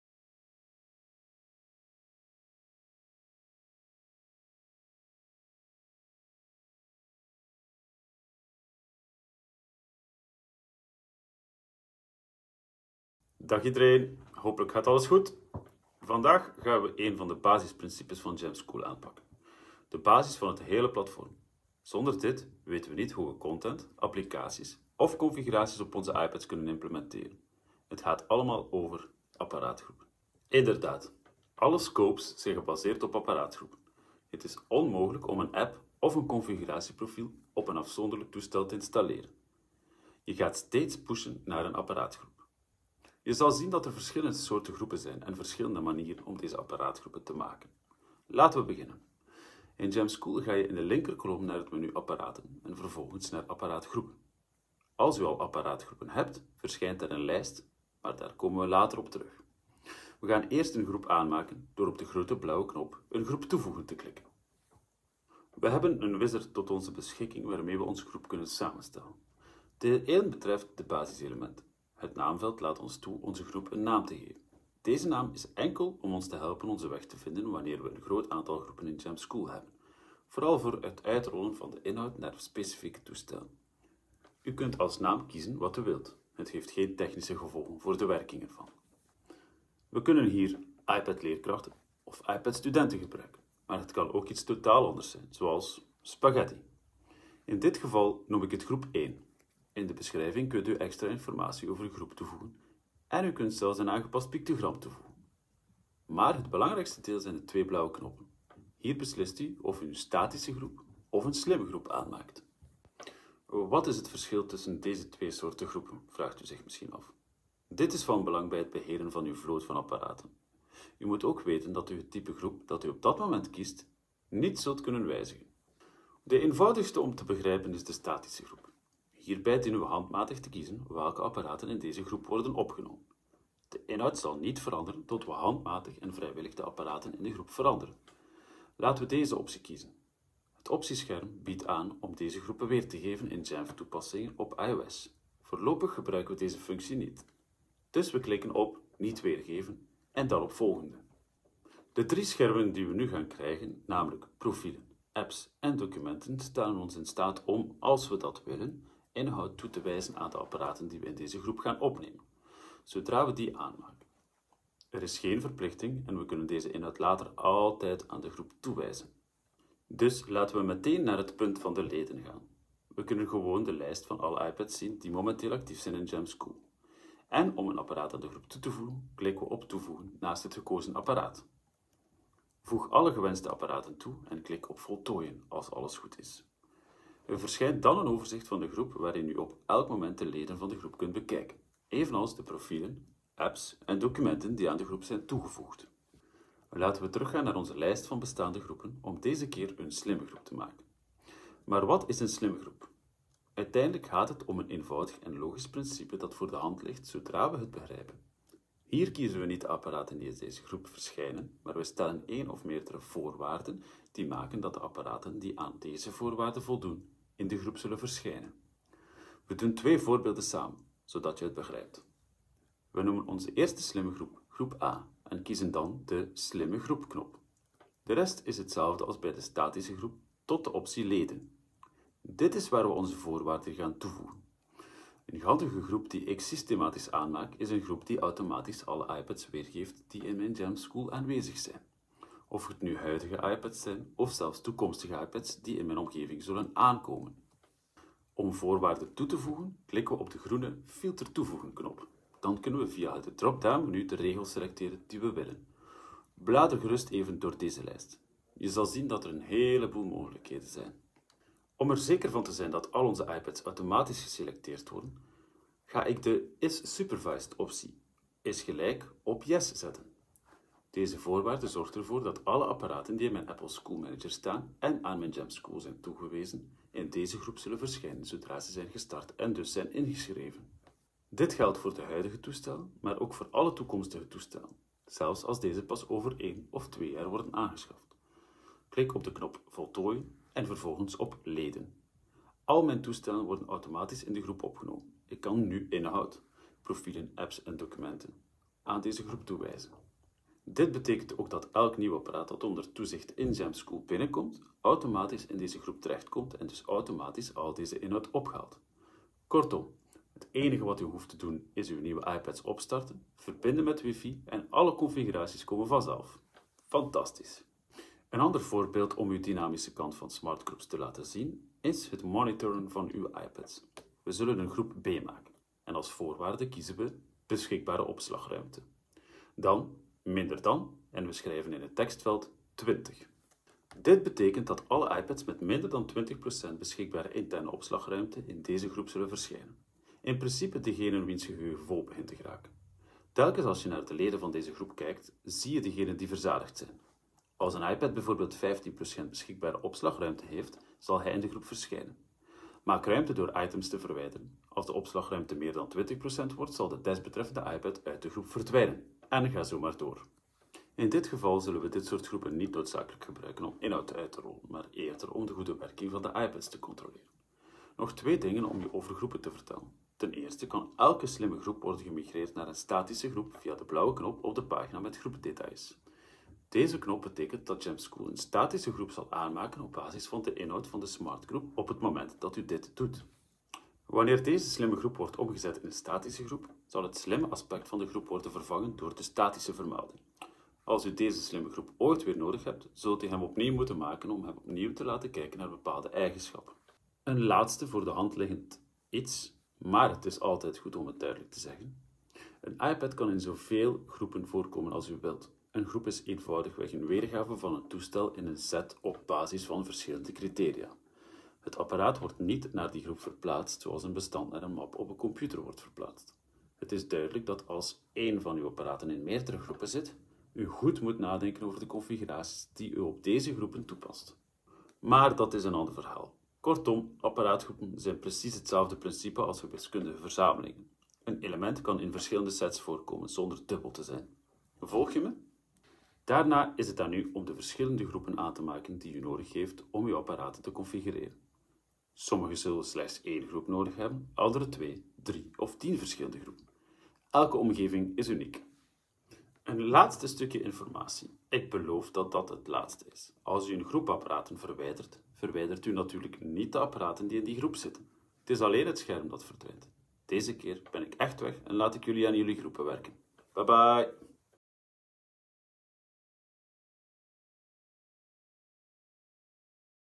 Econocom Onderwijsacademie Seizoen 1 - Aflevering 4: Apparaatgroepen in Jamf School
Apparaatgroepen vormen de basis voor het beheer van je iPads met Jamf School. Je verdeelt instellingen en inhoud immers niet over individuele apparaten, maar over apparaatgroepen.
In deze video leer je welke soorten apparaatgroepen er zijn, wat hun verschillen zijn en hoe je ze slim kan inzetten."